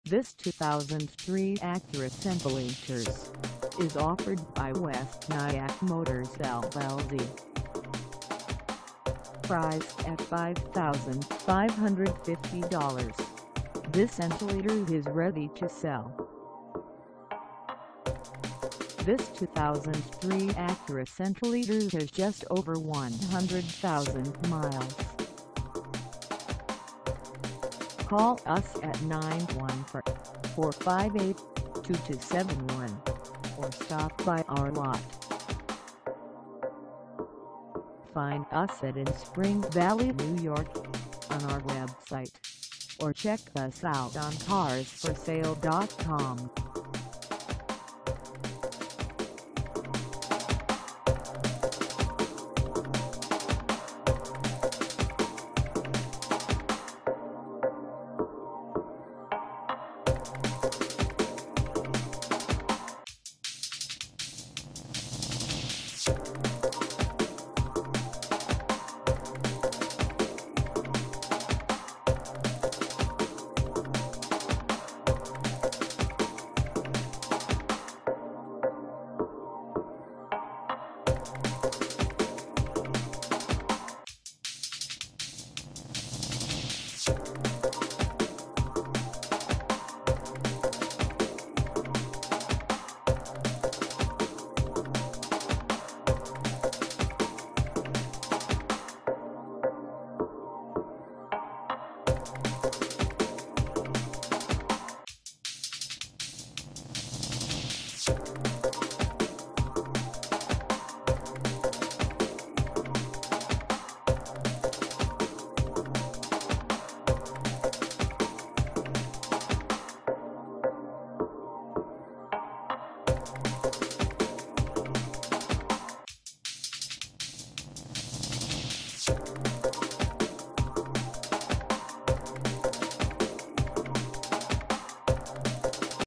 2003 Acura CL for sale in Spring Valley, NY 10977 at the Wes
This 2003 Acura CL is for sale in Spring Valley, NY 10977 at West Nyack Motors llc.
Engine: V6 3.2L V6 Natural Aspiration
Transmission: Automatic 5-Speed
Color: blue
Mileage: 100000
Contact West Nyack Motors llc  Spring Valley, NY 10977 to test drive this 2003 Acura CL  today.